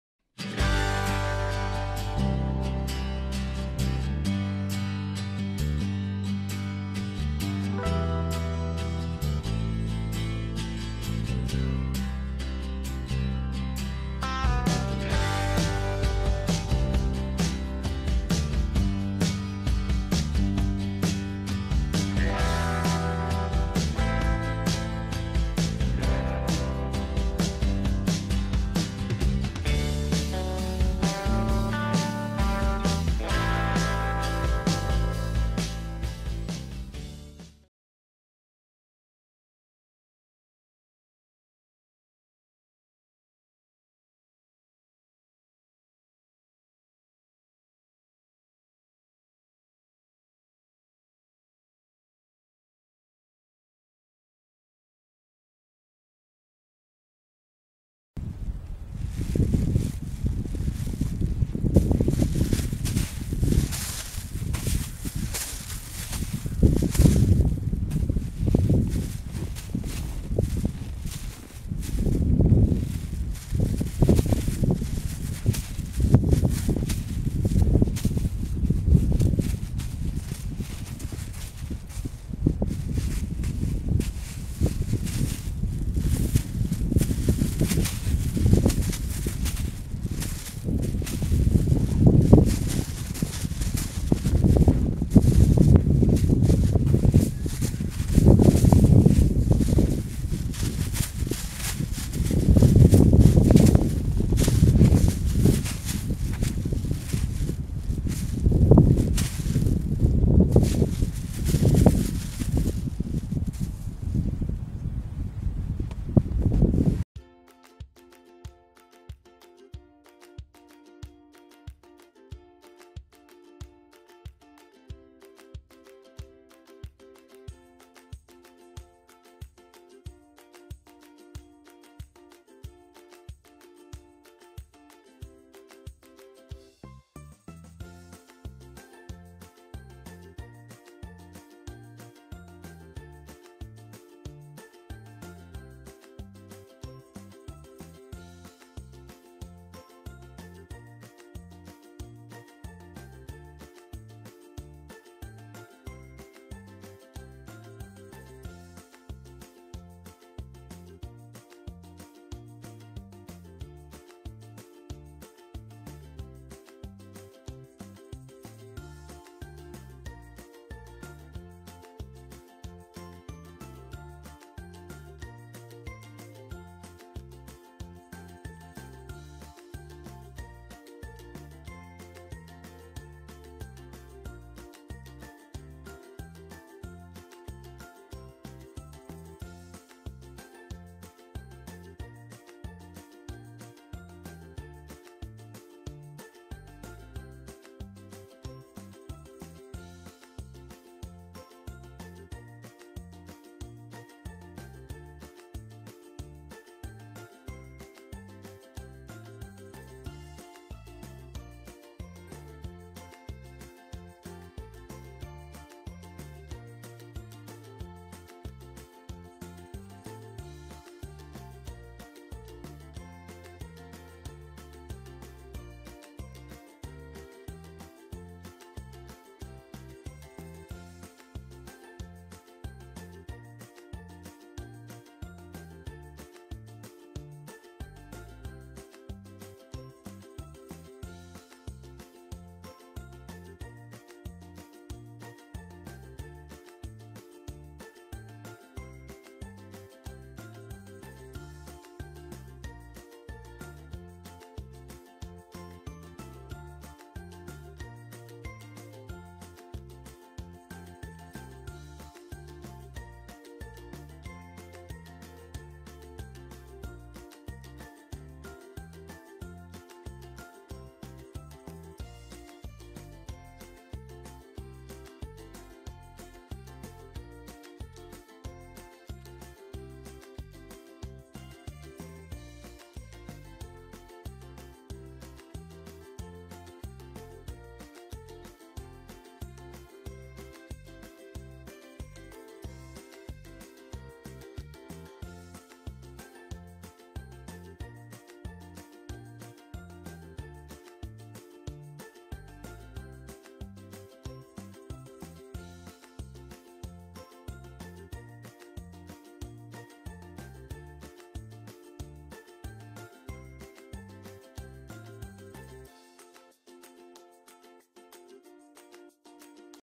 WH Midnight Traveler - Update - Plastic Bag and In Hand Trail
Just some clips from one of our training sessions with stud colt WH Midnight Traveler. He has grown in height, just waiting on him to fill out in anticipation of our Yearling APHA shows. Not doing too bad with the beginnings of his In Hand Trail training.
"Thunder" is 8 months old as of Feb 2018.